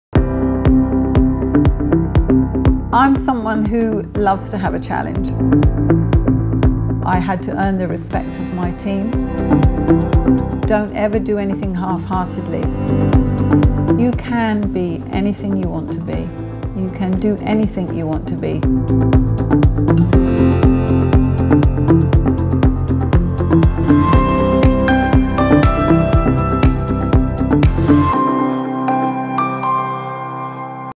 WomenOfSubstance -Debbie Stanford-Kristiansen Promo
Meet the lady who is elevating cinema experience in UAE, Debbie Stanford-Kristiansen - CEO at Novo Cinemas.